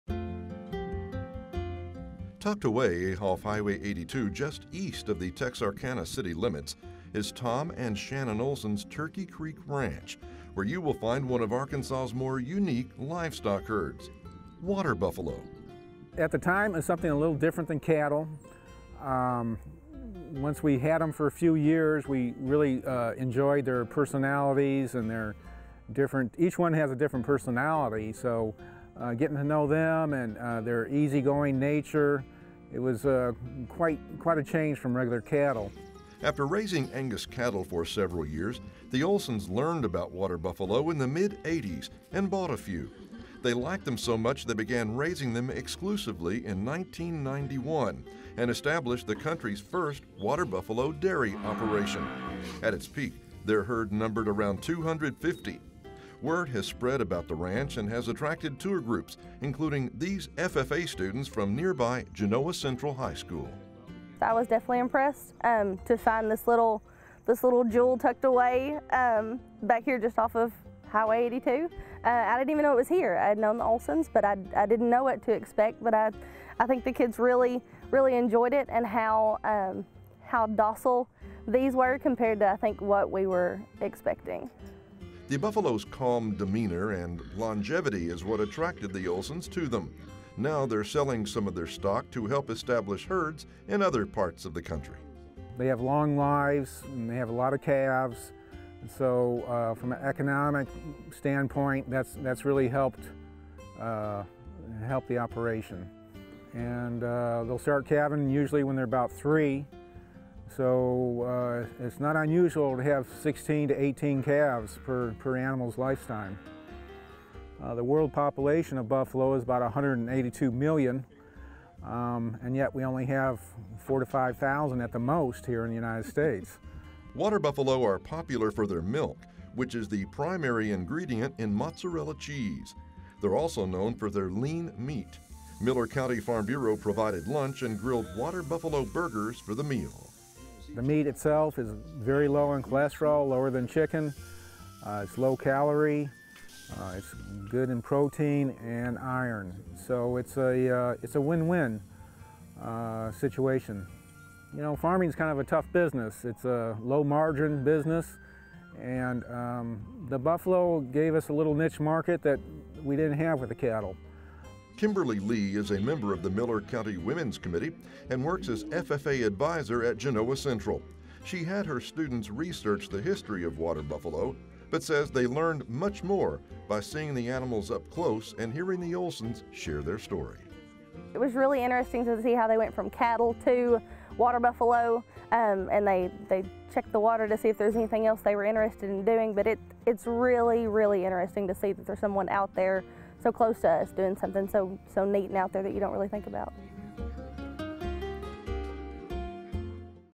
Raising Water Buffalos
Tucked away off highway 82 just east of the Texarkana city limits is Tom and Shannon Olson’s Turkey Creek Ranch where you will find one of Arkansas’ more unique livestock herds, Water Buffalo.